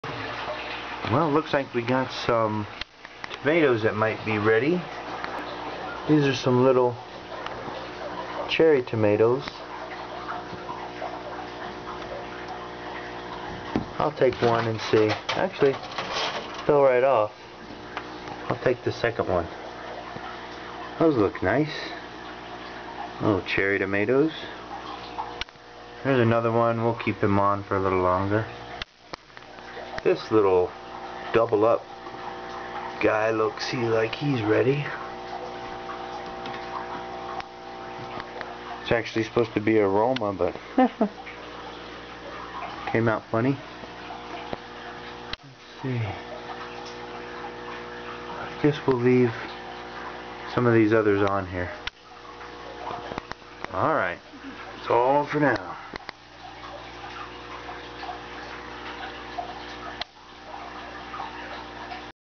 Organic Garden Set Up Aquaponic Cherry Tomatoes
Learn more about setting up your own organic aquaponic hydroponic garden with our online video guide, explanation and discussion board